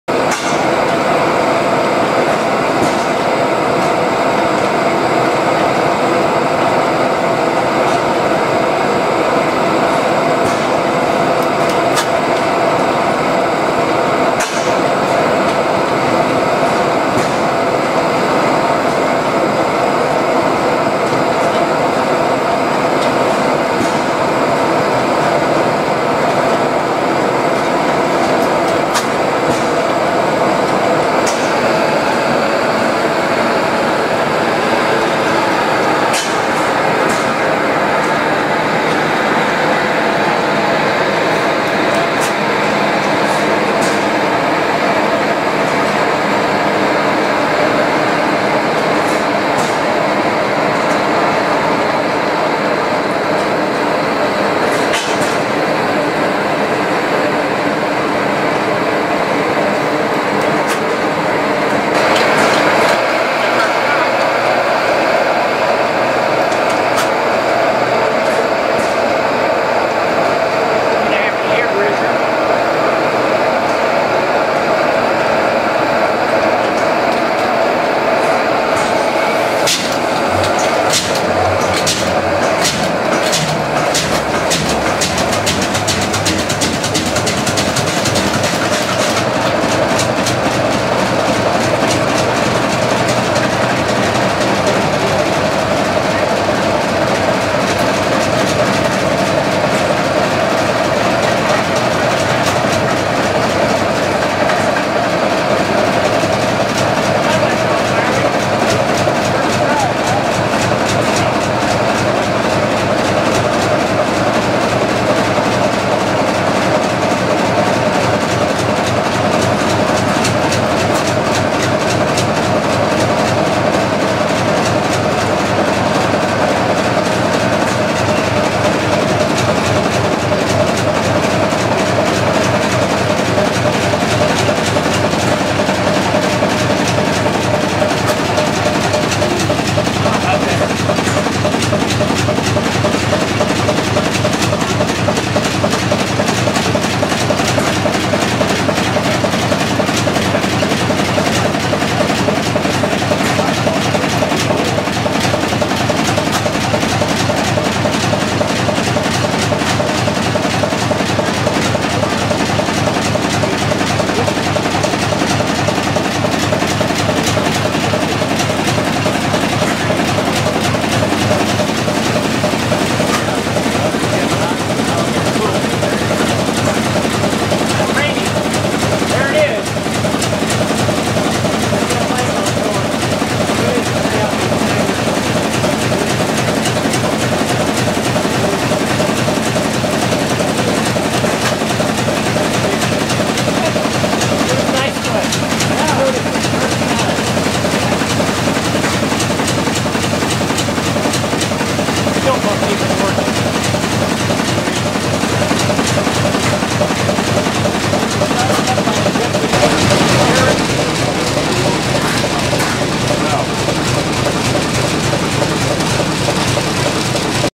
Air-Starting The Big 15,701 Cubic Inch 1922 Fairbanks-Morse Y-V Long Version 6-16-18
A big slug of compressed air is forced into the 5233.6-cubic inch cylinder on the right side of the engine to get things to start turning while the guy next to the engine acts as the fuel pump until the engine starts running on it's own at which point the pump is driven by the engine itself.

This 1922 three-cylinder 15,701-cubic inch Fairbanks-Morse Y-V is now preserved and operated at the Antique Gas & Steam Engine Museum (AGSEM) in Vista, California.  Shot on Saturday June 16th, 2018.